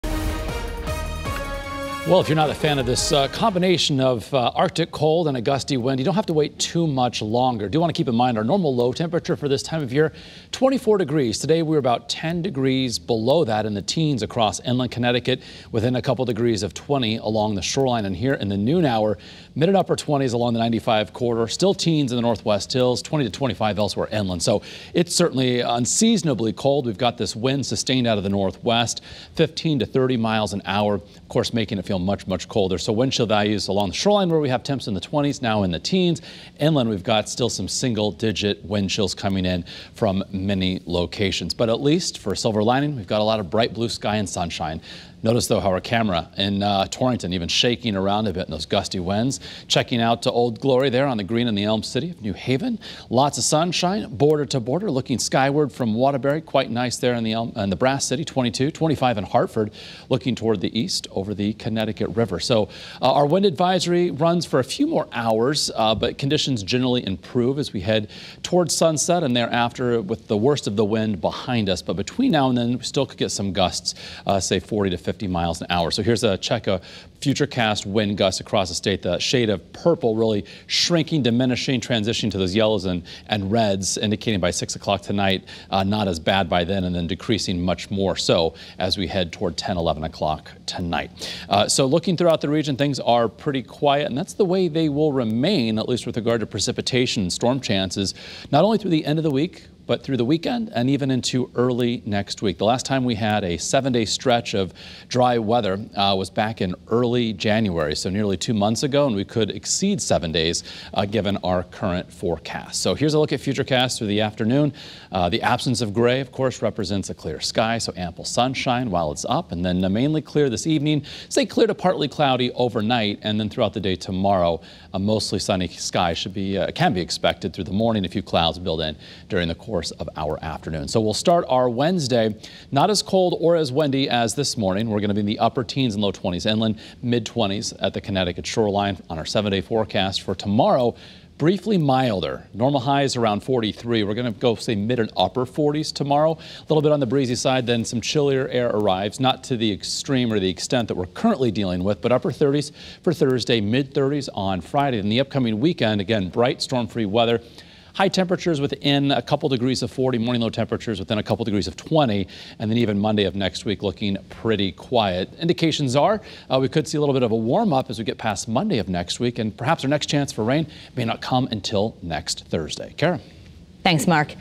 FORECAST: Wind to wind down throughout the day
Meteorologist Mark Dixon said the wind will wind down throughout the day. Here's his Tuesday noon forecast.